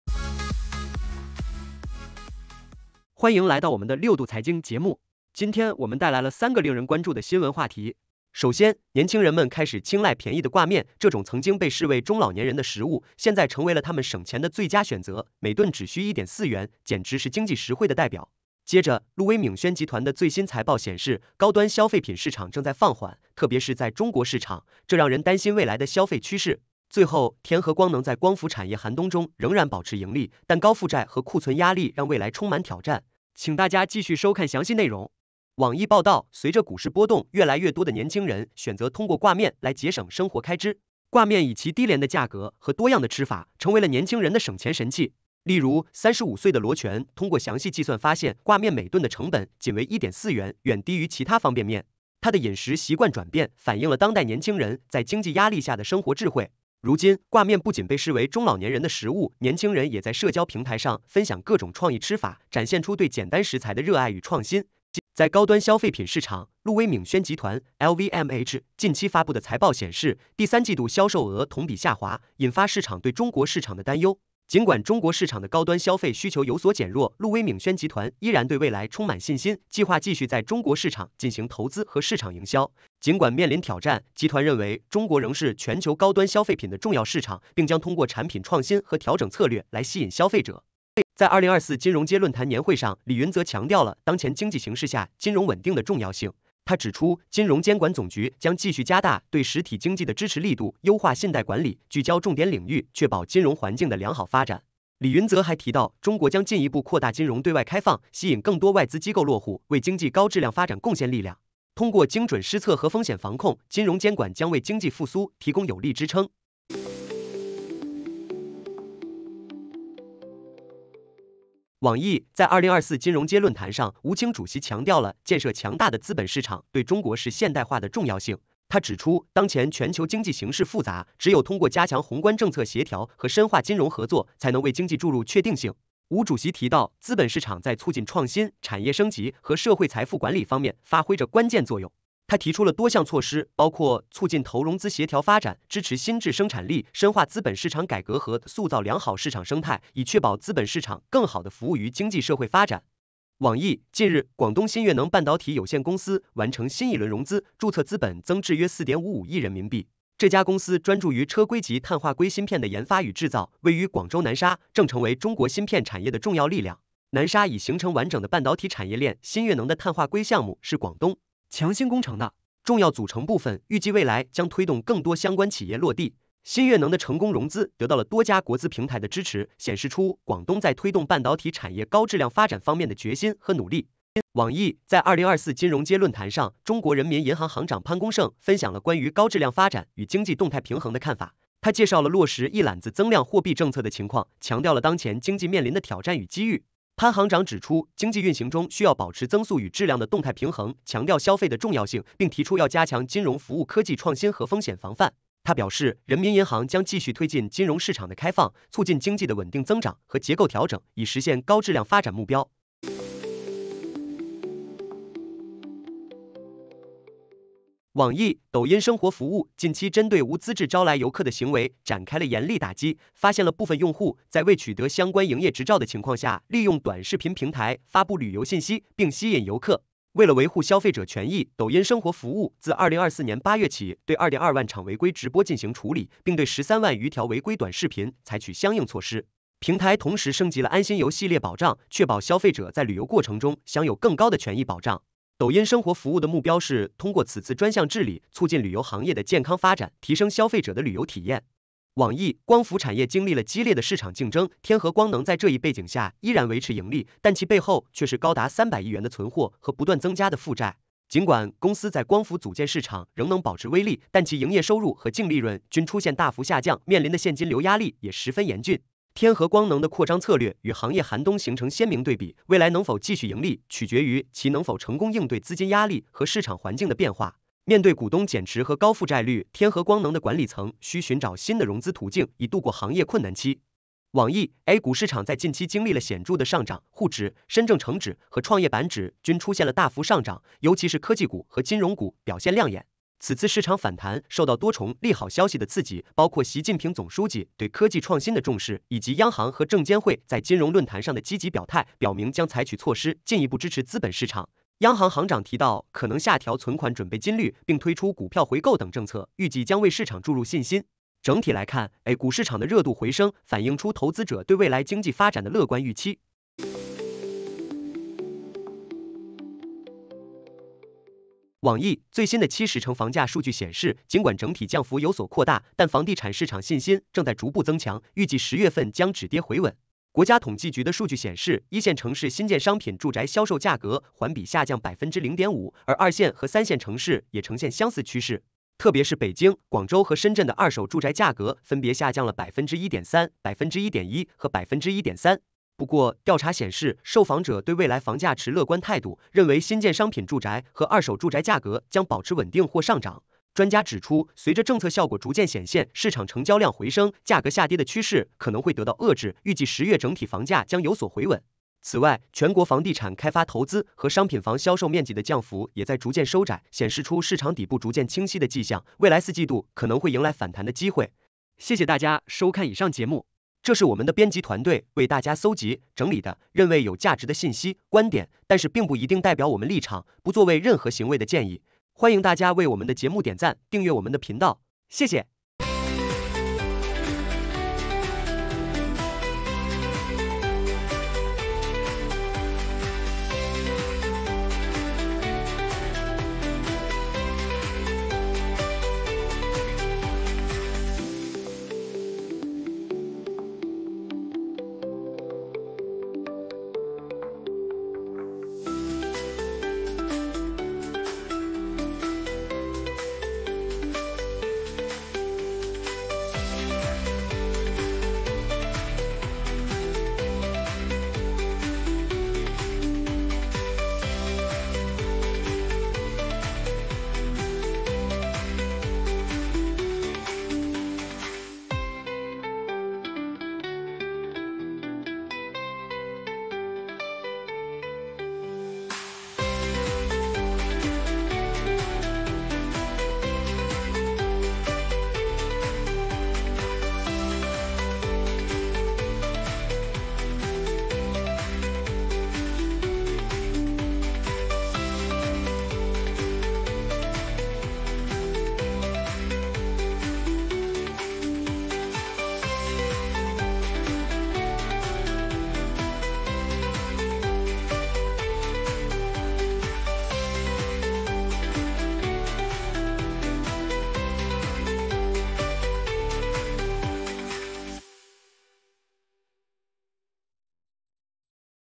挂面逆袭：年轻人省钱新宠；LVMH业绩下滑：高端消费品市场寒冬来袭；天合光能：光伏行业寒冬中的逆袭：六度財經20241018
歡迎來到我們的《六度財經》節目！今天我們帶來了三個令人關注的新聞話題。首先，年輕人們開始青睞便宜的掛面，這種曾經被視為中老年人的食物，現在成為了他們省錢的最佳選擇，每頓只需1.4元，簡直是經濟實惠的代表！接著，路威酩軒集團的最新財報顯示，高端消費品市場正在放緩，特別是在中國市場，這讓人擔心未來的消費趨勢。最後，天合光能在光伏產業寒冬中仍然保持盈利，但高負債和庫存壓力讓未來充滿挑戰。請大家繼續收看詳細內容！